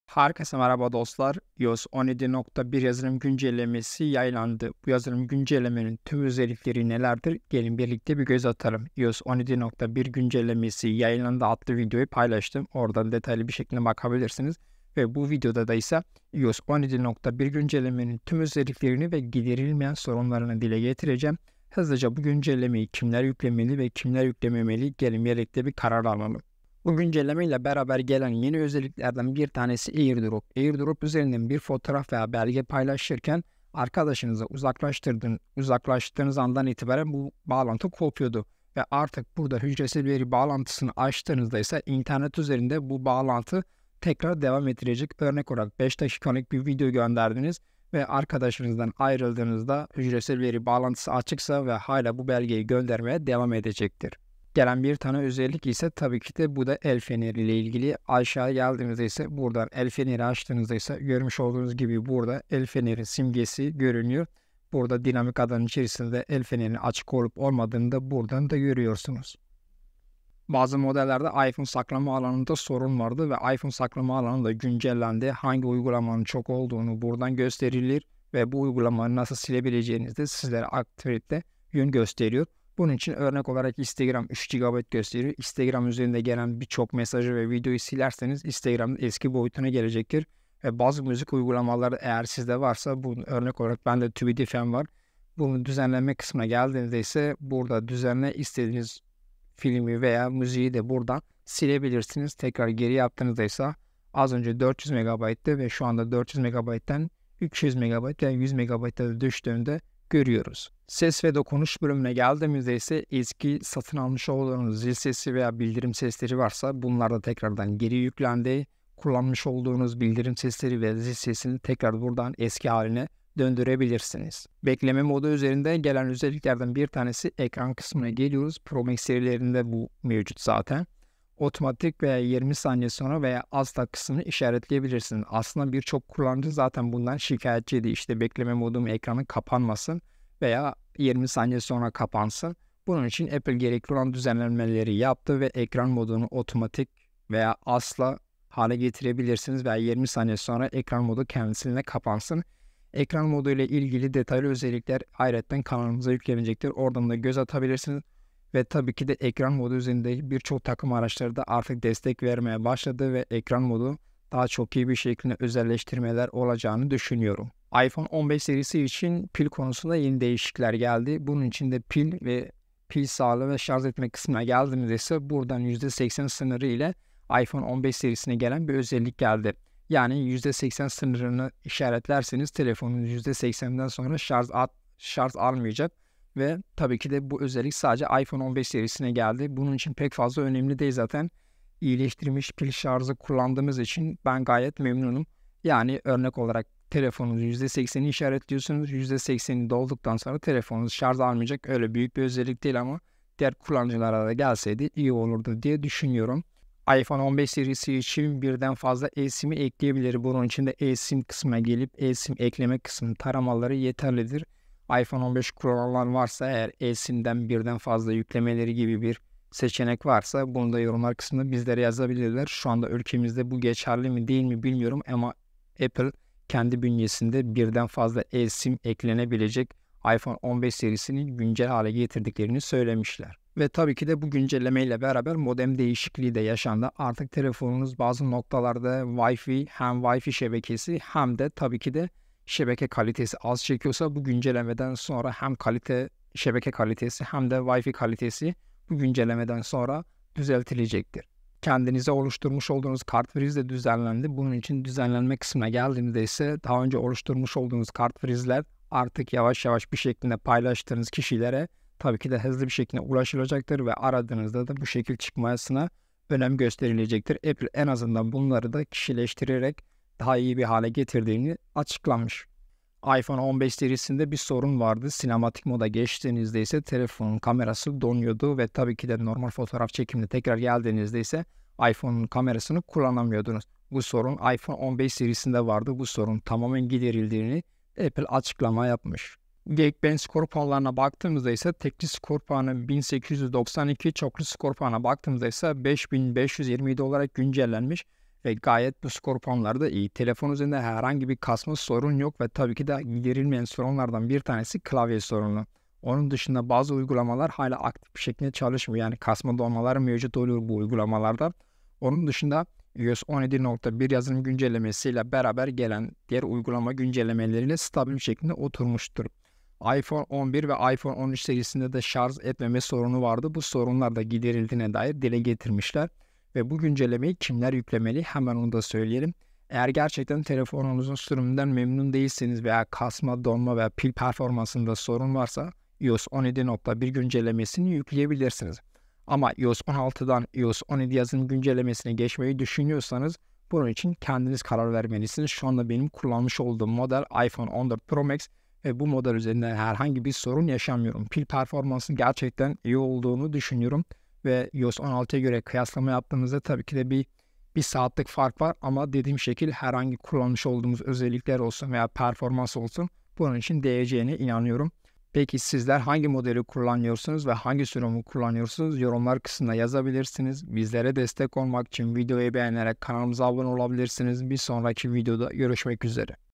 iOS 17.1 TÜM ÖZELLİKLERİ
iOS 17.1 yazılım güncellemesi yayınlandı bu güncelleme de hangi özellikler var iOS 17.1 tüm özellikleri Ekran moduna yeni gelen ve AirDrop hücresel bağlantı özelliği geldi dinamik ada içinde bir el feneri açtığınızda beli olacak iOS 17.1 tüm özelliklerini sizlerle paylaştım

Herkese merhaba ben Murat Ünal TEKNOLOJİ platformu